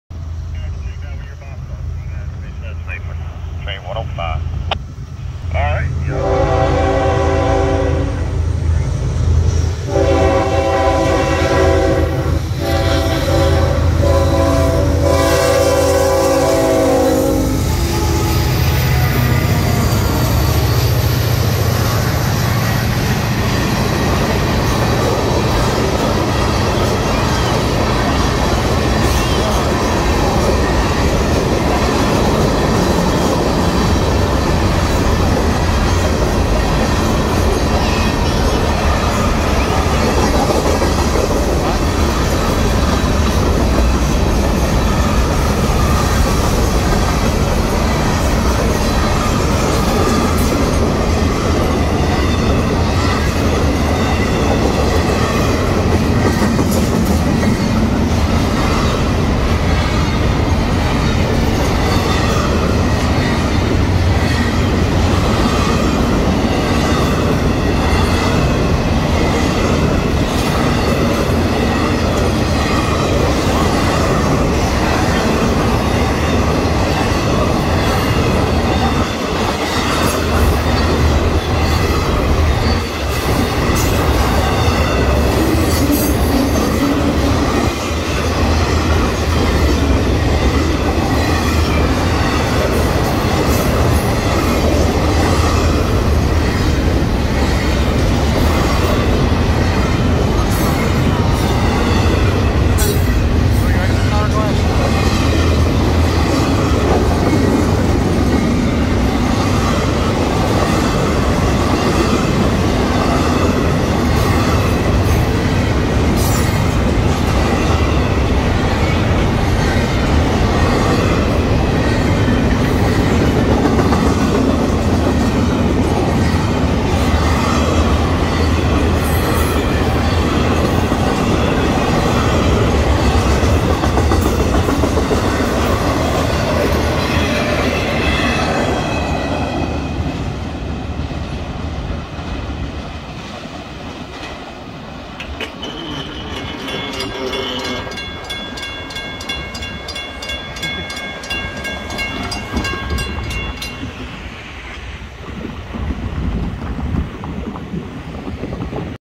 Florida East Coast Railway Train 105 at Pineda South - 8/28/21
Train 105 zooms by on the Pineda siding and passes train 206 waiting on the main track in the evening of Saturday August 28, 2021. Train 206 northbound had just come to a stop on the main track and Train 105 was cleared to pass on the siding. Video taken from behind the crossing gates at Suntree Blvd.  the 2nd track was installed on the crossing several months ago and they just recently laid the rails further south for the new track.